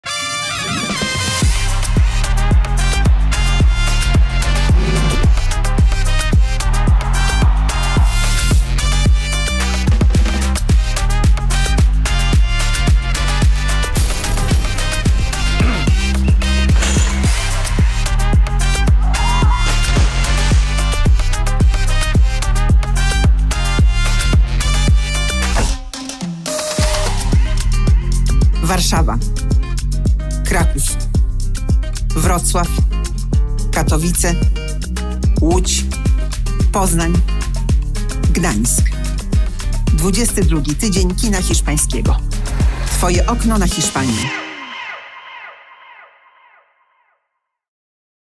22. Tydzień Kina Hiszpańskiego - spot (14-24 marca 2024)
22. Tydzień Kina Hiszpańskiego odbędzie w dniach 14-24 marca 2024 w siedmiu miastach w Polsce.